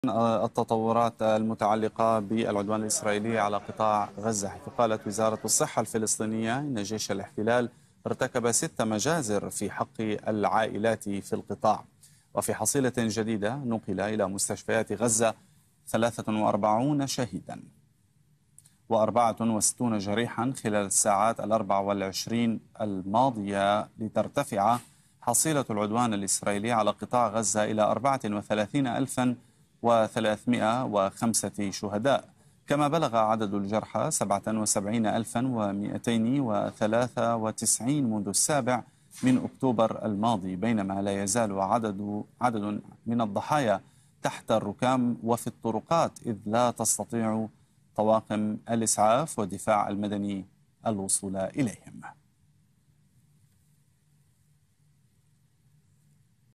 الاحتلال الإسرائيلي يرتكب 6 مجازر جديدة في آخر 24 ساعة وارتفاع حصيلة الشهداء والجرحى في غزة
أعلنت وزارة الصحة في غزة ارتفاع حصيلة العدوان الإسرائيلي على القطاع إلى 34305 شهداء و77293 مصابا من 7 أكتوبر مؤكدة ارتكاب الاحتلال الإسرائيلي 6 مجازر جديدة في آخر 24 ساعة.